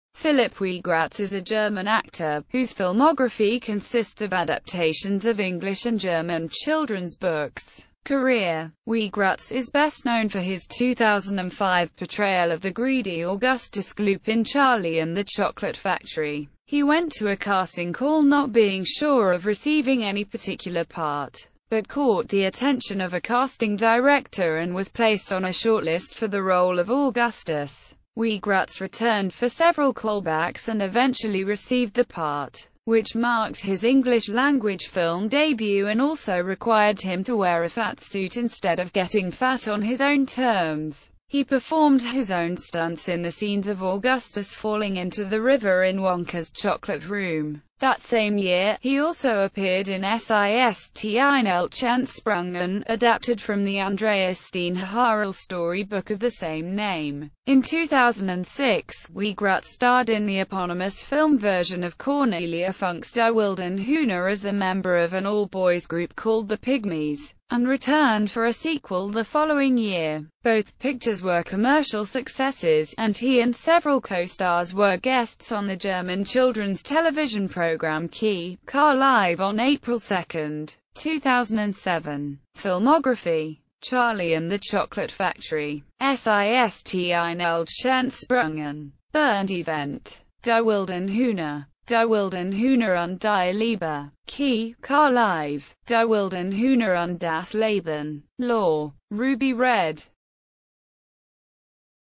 Philip Wiegratz
Philip Wiegratz (born February 17, 1993) is a German actor, whose filmography consists of adaptations of English and German children's books.